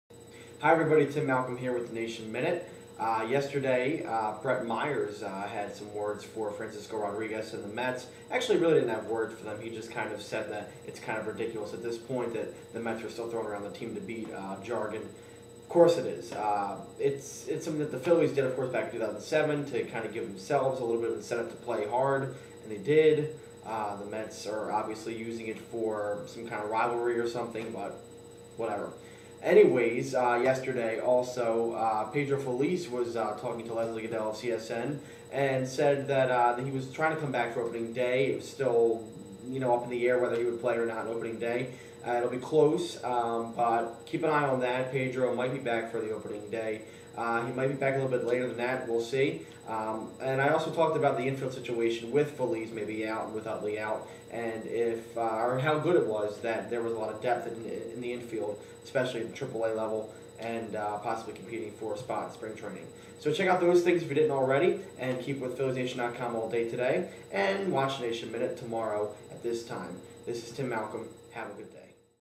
The Nation Minute, 2-17-08
Tim Malcolm of Philliesnation.com looks at:

- Brett Myers' response to Francisco Rodriguez
- Pedro Feliz's journey back to health
- The AAAA infield depth